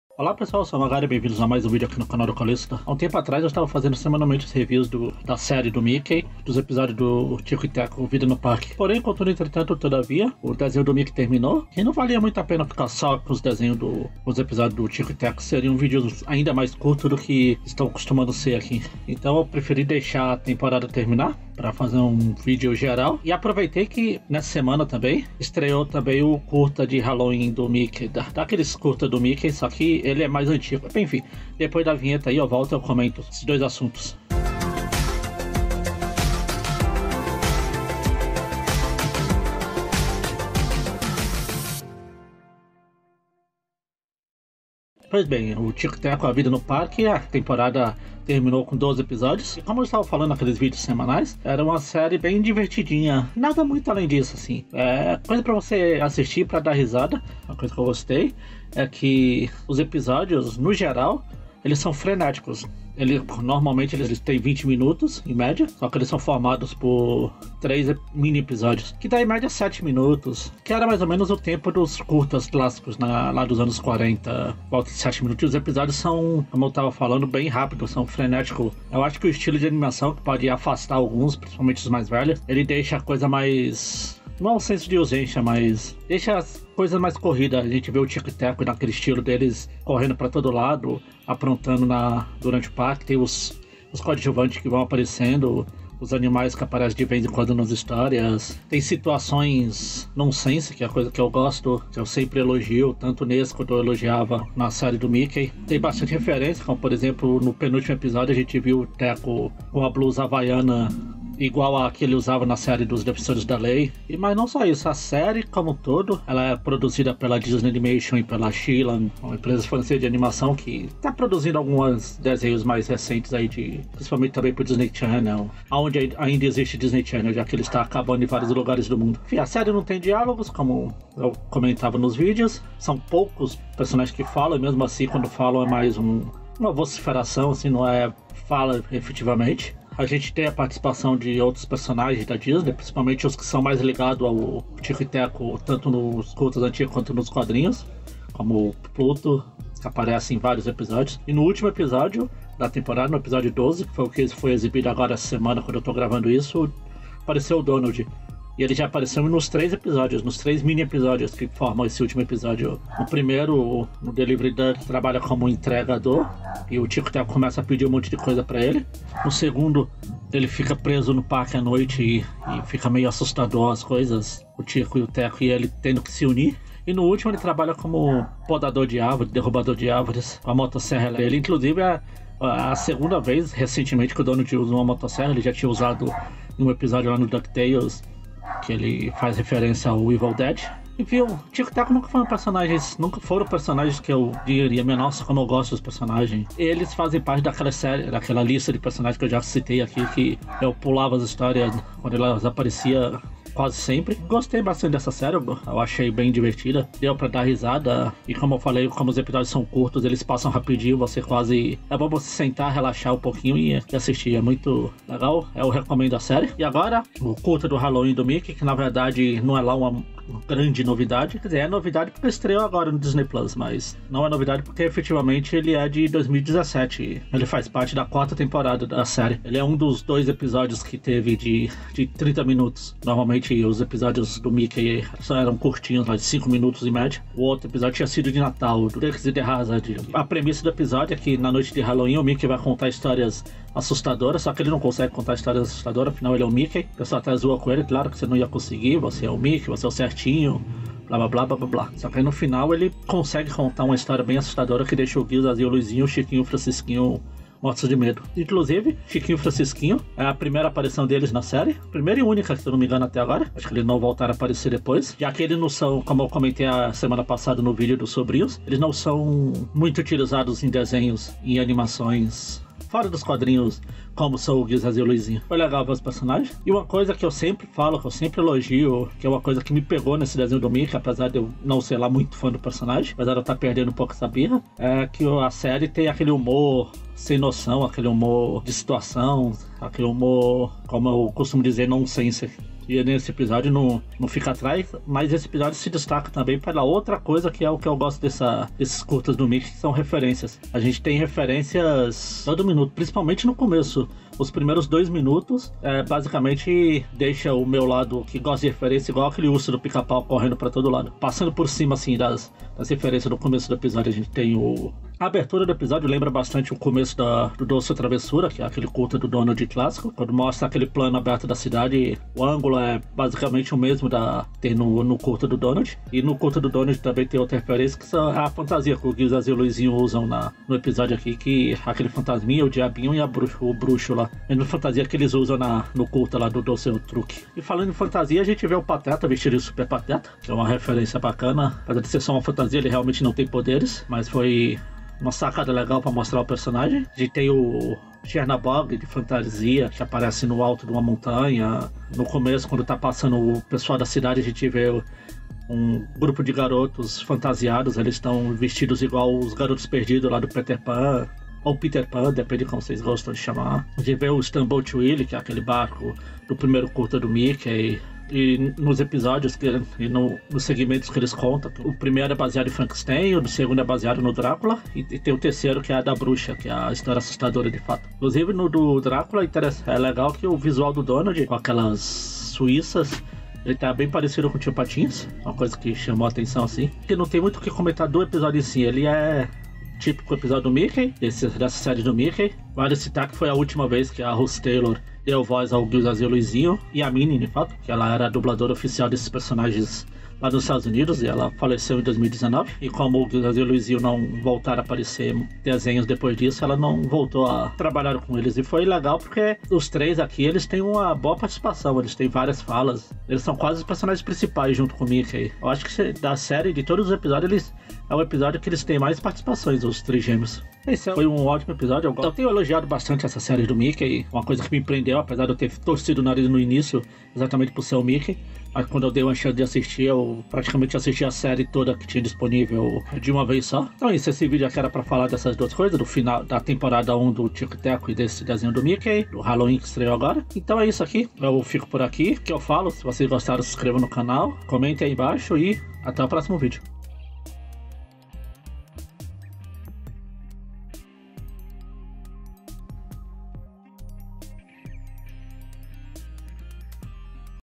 Calisota TV: Halloween do Mickey E Tico E Teco Vida No Parque.
No vídeo de hoje comento sobre a primeira temporada de Tico e Teco Vida no Parque, que já havia comentado um pouco antes além do especial de Halloween do Mickey que, apesar de ser de 2017, acabou de chegar no Disney Plus.